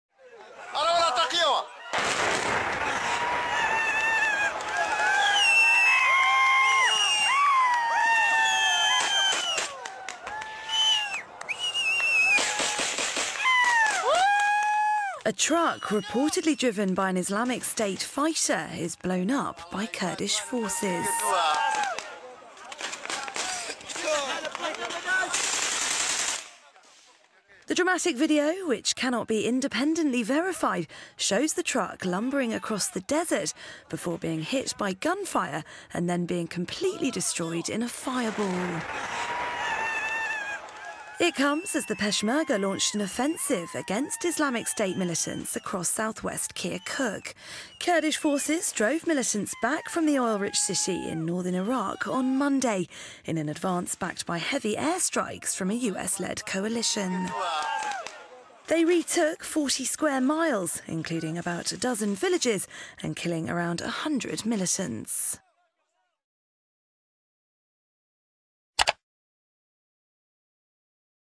Dramatic video: Islamic State truck blown up near Kirkuk
A heavily armoured truck reportedly being driven by an Islamic State combatant is blown up by Kurdish fighters. Report by Sophie Foster.